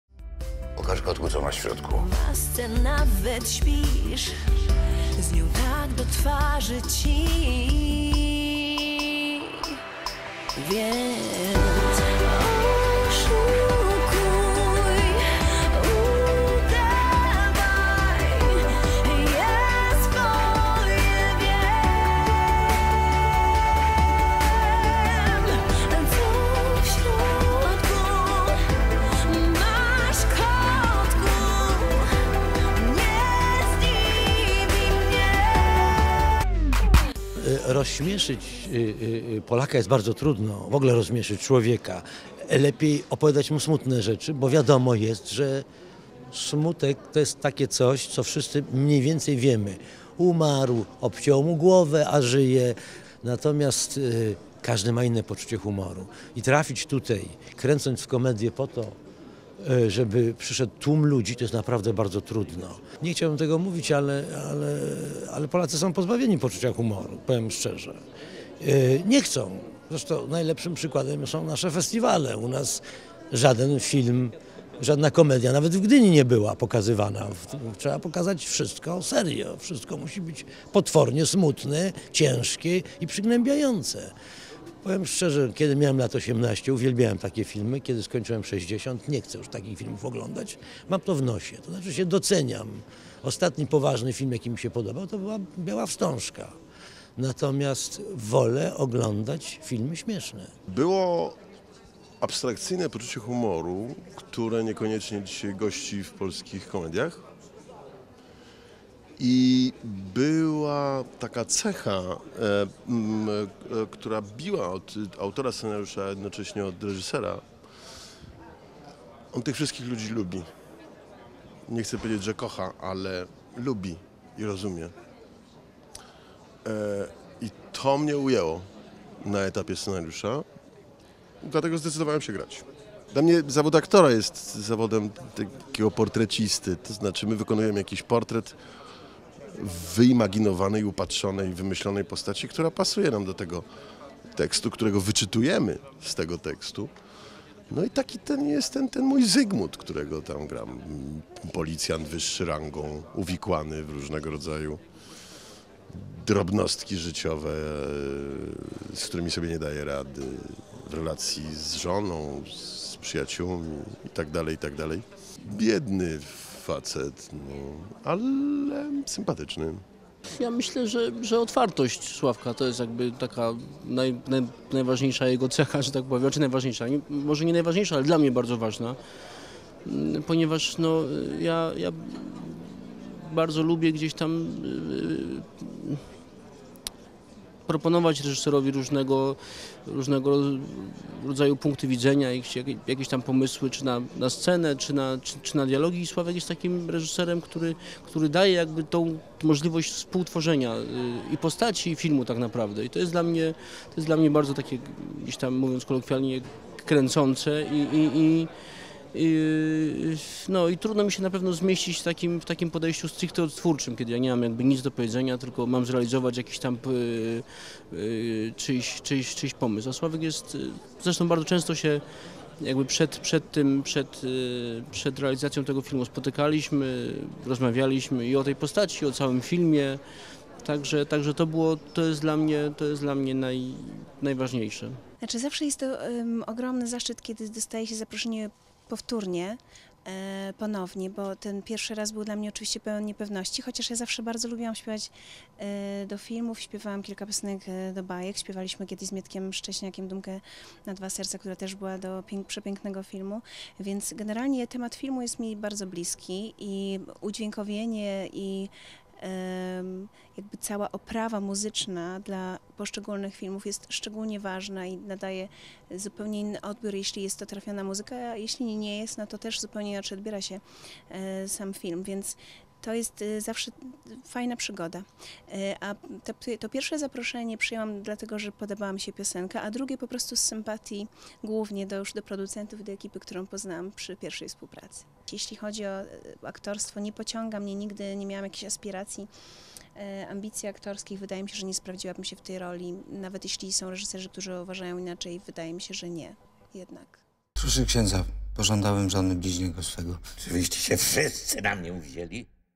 Premiera filmu "Pokaż kotku, co masz w środku"
W warszawskim Multikinie Złote Tarasy odbyła się uroczysta premiera komedii "Pokaż kotku, co masz w środku". Nasza ekipa miała okazję porozmawiać z reżyserem Sławomirem Kryńskim, aktorami Krzysztofem Stelmaszykiem i Jackiem Borusińskim oraz Edytą Górniak, która wykonała piosenkę do filmu.